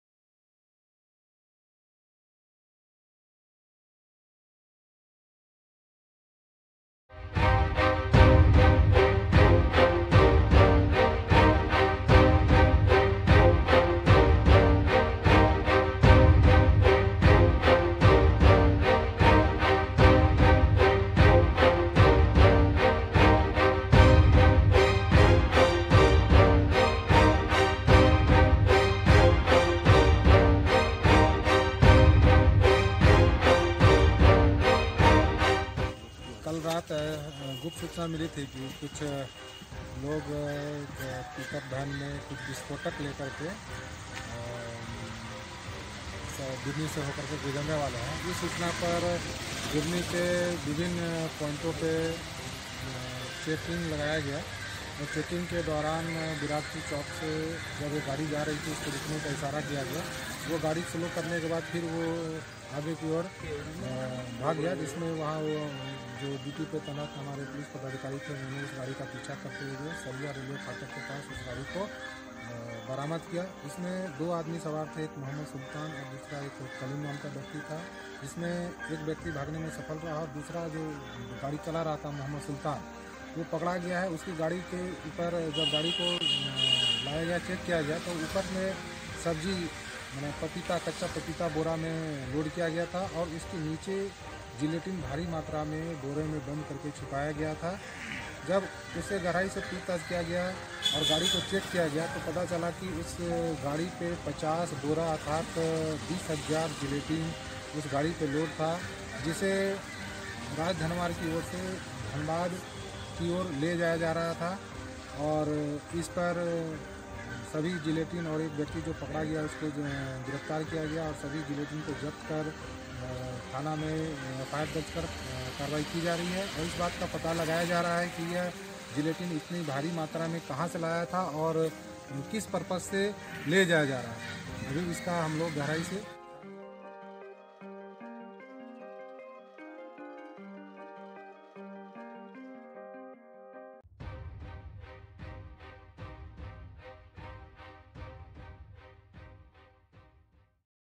गिरिडीह में पुलिस ने जब्त किया 20 हजार जिलेटिन
विधानसभा चुनाव से पहले गिरिडीह पुलिस के हाथ लगा बड़ी कामयाबी. पुलिस ने बरामद की 20 हजार जिलेटिन समेत भारी मात्रा में विस्फोटक सामग्री... पुलिस ने बिरनी थानाक्षेत्र के जंगलों से गुप्त सूचना के आधार पर की थी छापेमारी...